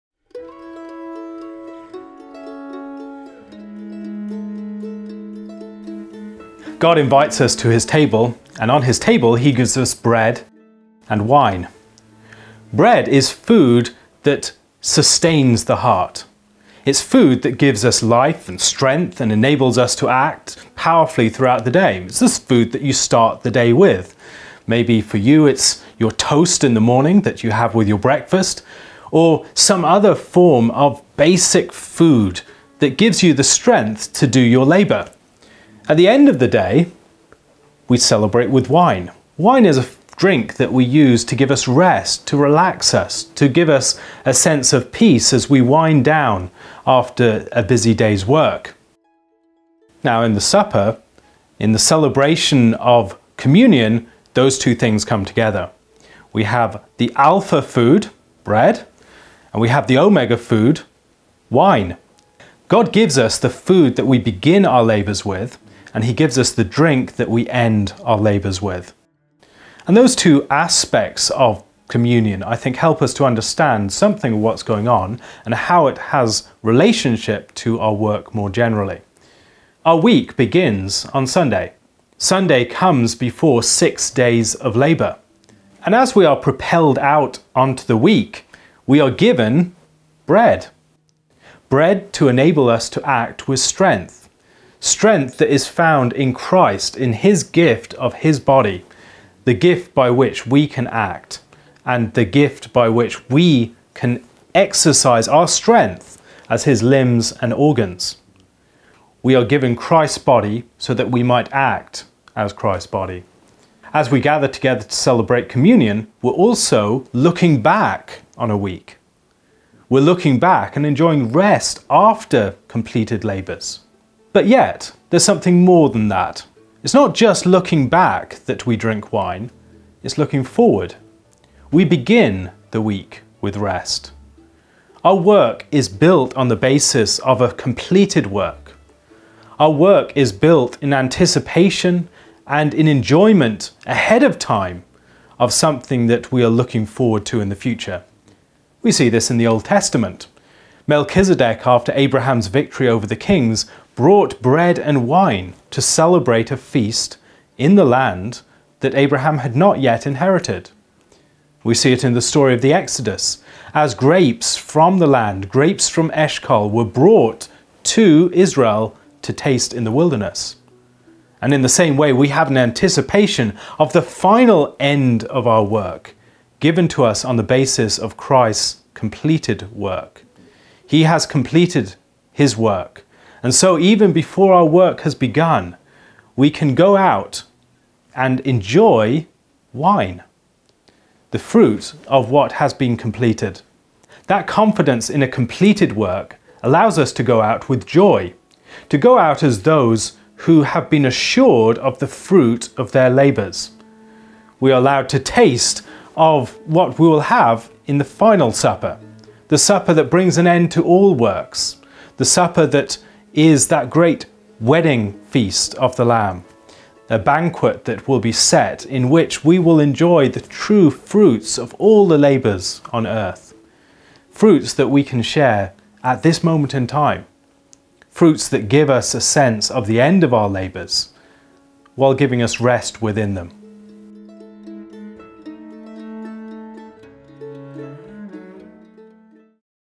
Alpha and Omega Food, with Alastair Roberts
Continuing our series on Liturgy and Work, Alastair Roberts discusses bread, wine, and what they mean for how we think about work and rest.

Learn More!